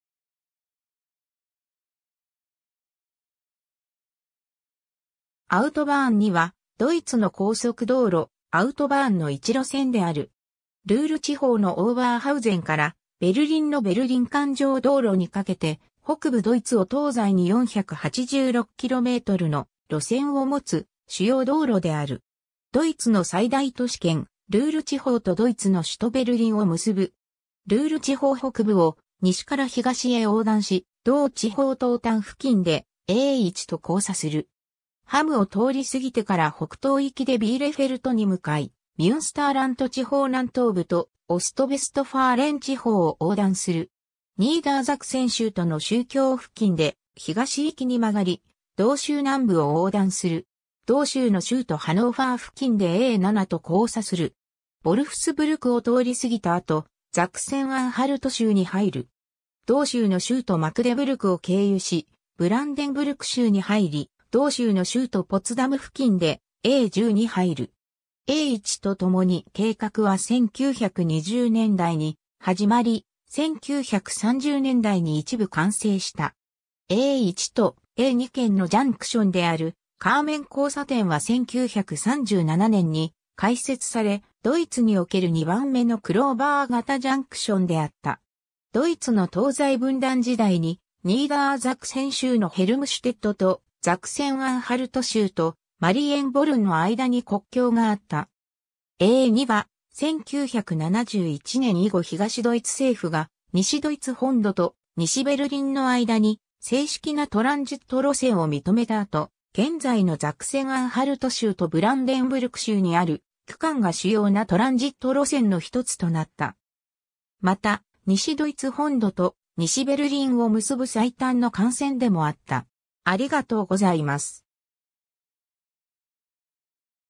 アウトバーン 2
ノルトライン＝ヴェストファーレンの交通
ザクセン＝アンハルトの交通
アウトバーン 2（独：Bundesautobahn 2、BAB 2 または A 2）はドイツの高速道路、アウトバーンの一路線である。
 ルール地方のオーバーハウゼンからベルリンのベルリン環状道路（Berliner Ring、アウトバーン 10）にかけて、北部ドイツを東西に486kmの路線を持つ主要道路である。
ドイツの最大都市圏、ルール地方とドイツの首都ベルリンを結ぶ。
ルール地方北部を西から東へ横断し、同地方東端付近でA1と交差する。
ハムを通り過ぎてから北東行きでビーレフェルトに向かい、ミュンスターラント地方南東部とオストヴェストファーレン（東ヴェストファーレン）地方を横断する。
ニーダーザクセン州との州境付近で東行きに曲がり、同州南部を横断する。
同州の州都ハノーファー付近でA7と交差する。
ヴォルフスブルクを通り過ぎた後、ザクセン＝アンハルト州に入る。
同州の州都マクデブルクを経由し、ブランデンブルク州に入り、同州の州都ポツダム付近でA10に入る。
 A 1とともに計画は1920年代に始まり、1930年代に一部完成した。
A 1とA 2間のジャンクションであるカーメン交差点（Kamener Kreuz）は1937年に開設され、ドイツにおける2番めのクローバー型ジャンクションであった。
 ドイツの東西分断時代にニーダーザクセン州のヘルムシュテットとザクセン＝アンハルト州とマリエンボルンの間に国境があった。
A2は、1971年以後東ドイツ政府が西ドイツ本土と西ベルリンの間に正式なトランジット路線を認めた後、現在のザクセン＝アンハルト州とブランデンブルク州にある区間が主要なトランジット路線の一つとなった。
また、西ドイツ本土と西ベルリンを結ぶ最短の幹線でもあった。